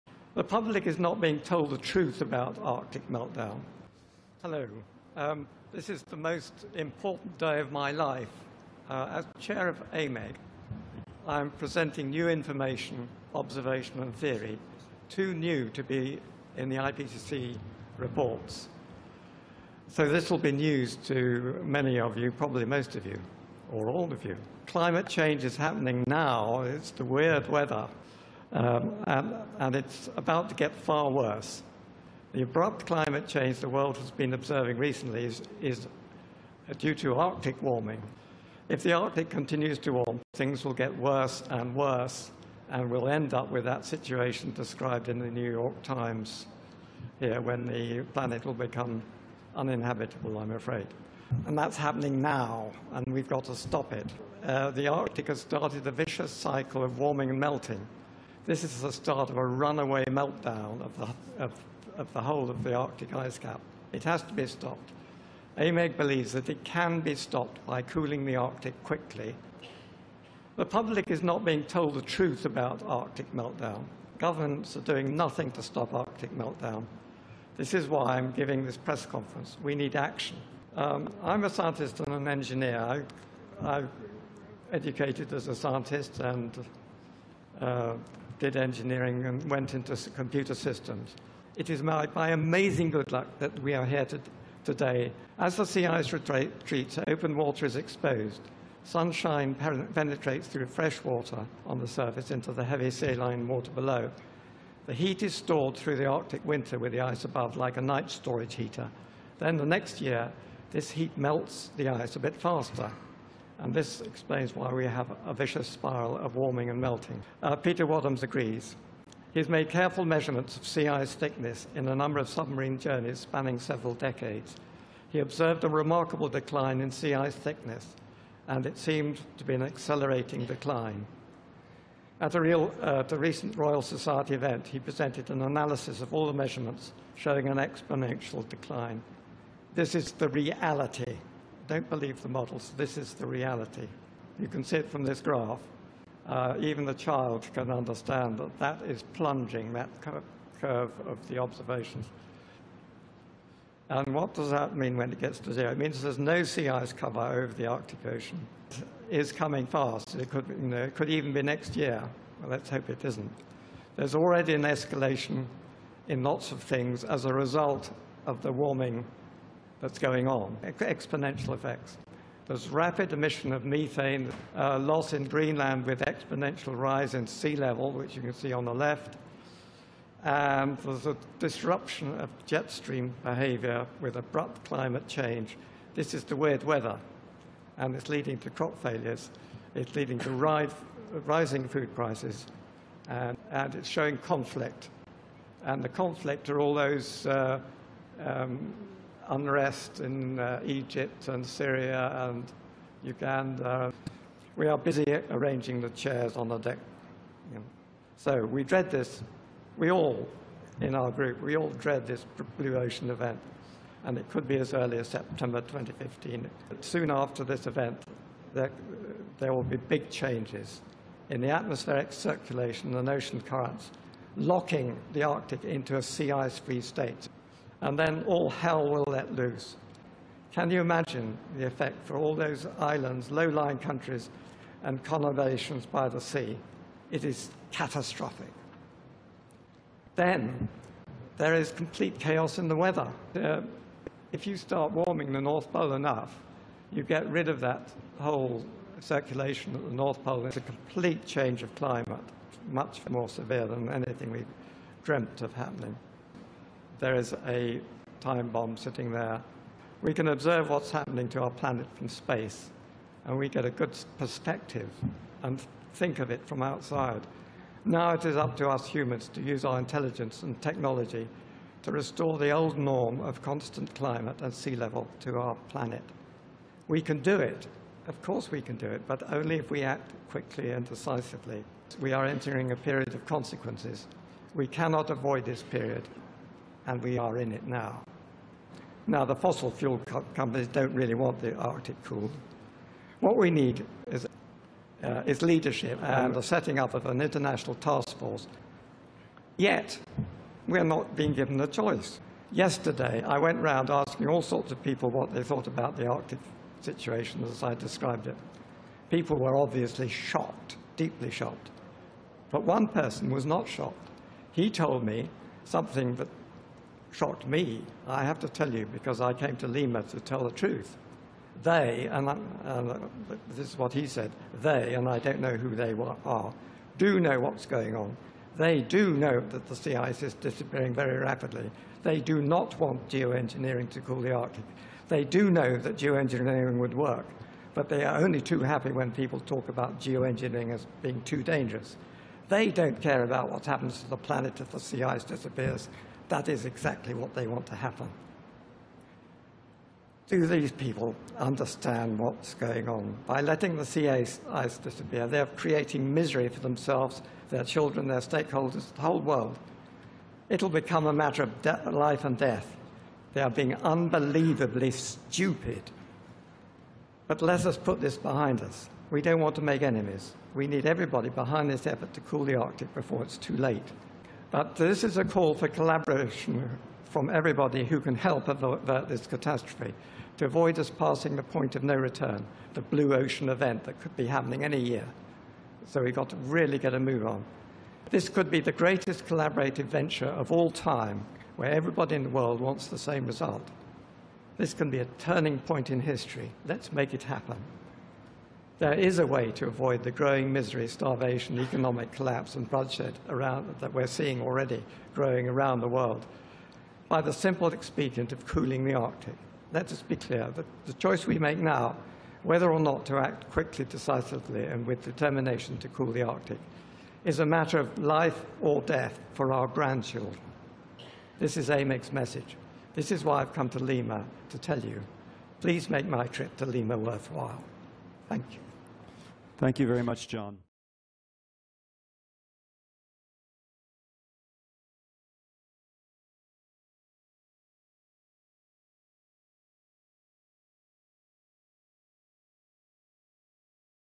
The Arctic Methane EMERGENCY
John Nissen, chair of the Arctic Emergency Methane Group (AMEG), talks about the catastrophic dangers that the warming of the Arctic region poses to modern civilization and the possible extinction of humans due to abrupt climate change. This press briefing took place during the 20th annual Conference of the Parties (COP 20) held on Dec. 5, 2014 for the United Nation's Framework Convention on Climate Change (UNFCCC) held in Lima, Peru.